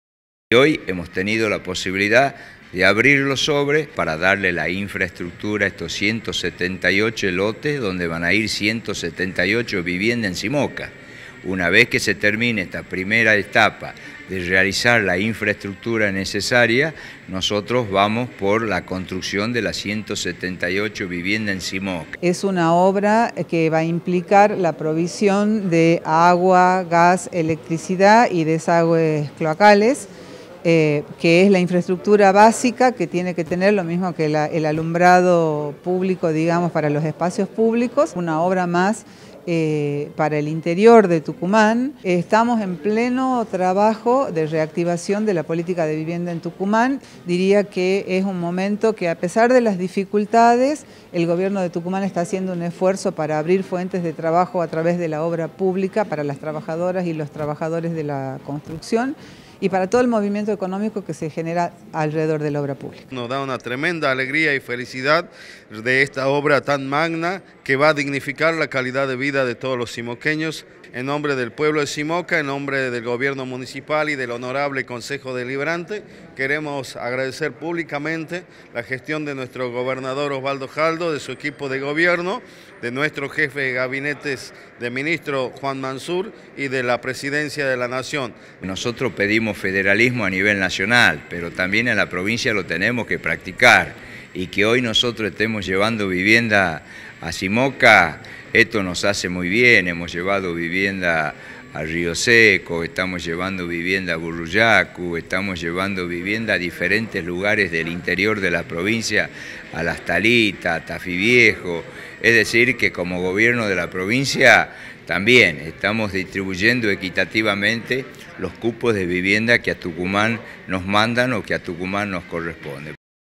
Osvaldo Jaldo | Comenzarán la construcción de infraestructura para un barrio en Simoca
𝗖𝗮𝘀𝗮 𝗱𝗲 𝗚𝗼𝗯𝗶𝗲𝗿𝗻𝗼 | Dimos otro importante paso en materia de soluciones habitacionales, realizando la apertura de sobres para las obras de infraestructura donde se construirán 178 viviendas en Simoca.

🏘️ Esto se da en el marco del programa Casa Propia, por el que vamos a construir 3.600 viviendas en Tucumán con apoyo federal.

🏗️ Las obras a llevarse cabo cuentan con un presupuesto oficial de más de $179 millones, los que se financian con recursos provinciales. Y demandarán un plazo de ejecución de 540 días.

🚧 En esta primera etapa se instalarán los servicios de agua potable, gas natural, cloacas, entre otros, a los 178 lotes. Una vez finalizada iniciaremos la construcción de las casas.

🤝🏻 Es importante destacar que el federalismo que pedimos a nivel nacional, lo estamos aplicando también en la provincia. Que hoy estemos llevando viviendas a Simoca implica llegar al interior. Como Estado estamos distribuyendo equitativamente los cupos de vivienda que a Tucumán le corresponden.

👥 Estuvieron presentes el vicepresidente de la Legislatura, Regino Amado; la secretaria general del Gobernación, Silvia Pérez; la interventora del Instituto Provincial de Vivienda y Desarrollo Urbano (IPVyDU), Stella Maris Córdoba; el ministro de Obras y Servicios Públicos, Fabián Soria; el ministro de Educación, Juan Pablo Lichtmajer; y el intendente de Simoca, Marcelo Herrera.